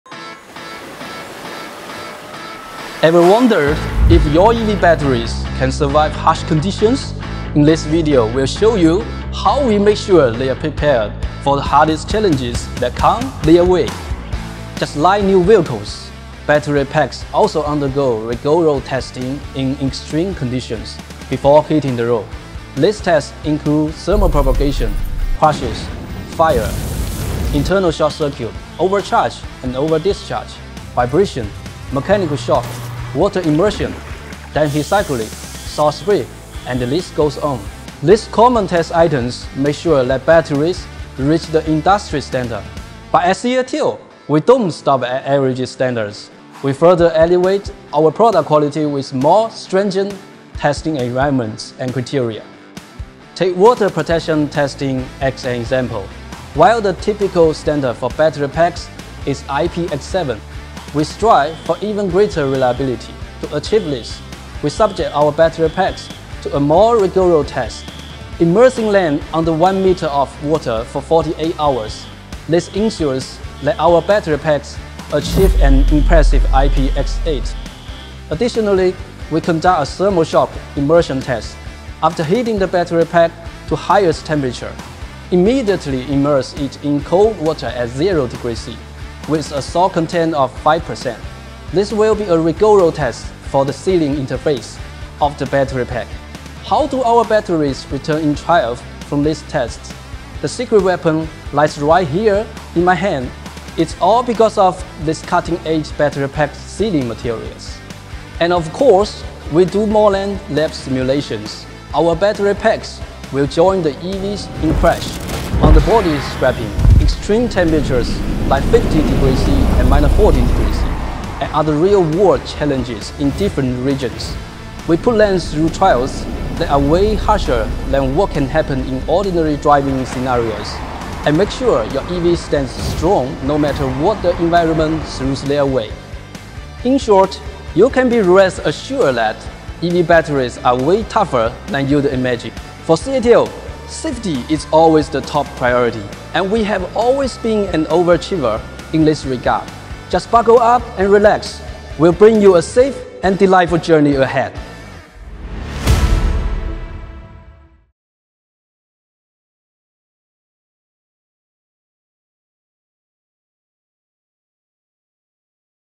Flames to Frost: How CATL Battery Braves It All
Concerned about your EV batteries faltering under tough weather conditions? Fear not! CATL's batteries are engineered to conquer the hardest challenges!

Armed with cutting-edge materials and technologies, our batteries withstand fire, water, shock, crushing forces, and extreme cold and heat, emerging unscathed to ensure top-tier reliability!